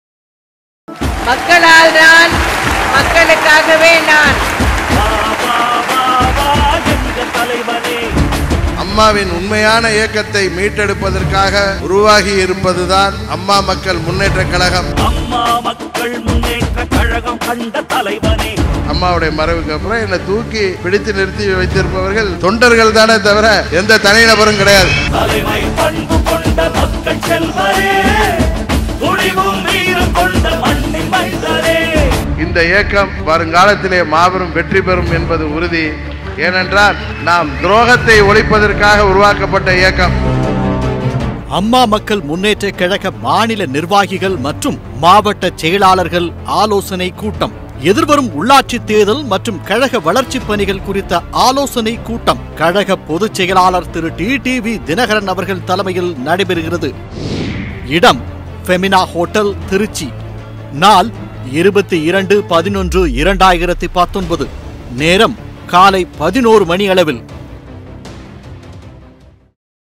உள்ளாட்சி தேர்தல் குறித்த தலைமைக்கழக நிர்வாகிகள் ஆலோசனைக் கூட்டம்!
உள்ளாட்சி தேர்தல் மற்றும் கழக வளர்ச்சிப் பணிகள் குறித்த தலைமைக்கழக நிர்வாகிகள், மாவட்ட கழக செயலாளர்கள் ஆலோசனைக் கூட்டம்!